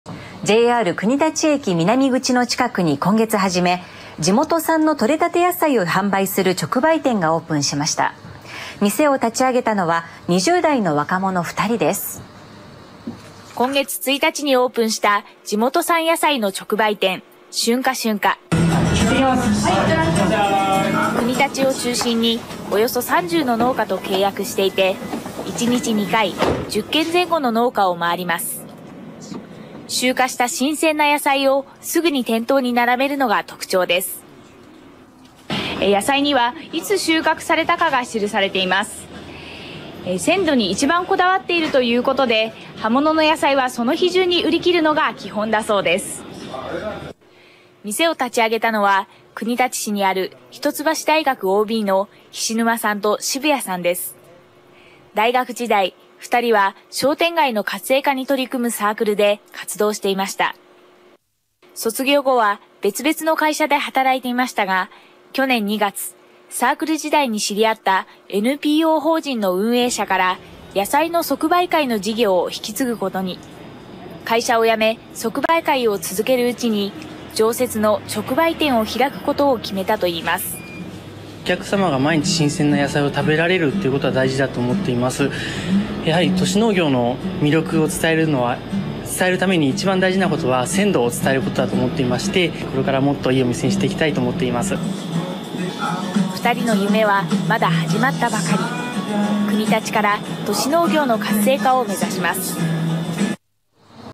JR国立駅南口にオープン 直売店で地元産野菜の魅力発信
JR国立駅南口の近くに今月初め、地元産のとれたて野菜を販売する直売店がオープンしました。店を立ち上げたのは20代の若者2人です。
　今月1日にオープンした地元産野菜の直売店『しゅんかしゅんか』は国立を中心におよそ30の農家と契約していて、1日2回、10軒前後の農家を回ります。集荷した新鮮な野菜をすぐに店頭に並べるのが特徴で、野菜にはいつ収穫されたかが記されています。鮮度に一番こだわっているため、葉物の野菜はその日のうちに売り切るのが基本だということです。
　この店を立ち上げたのは国立市にある一橋大学OBの菱沼勇介さんと渋谷祐輔さんです。大学時代に2人は商店街の活性化に取り組むサークルで活
■■↓全文を見る■■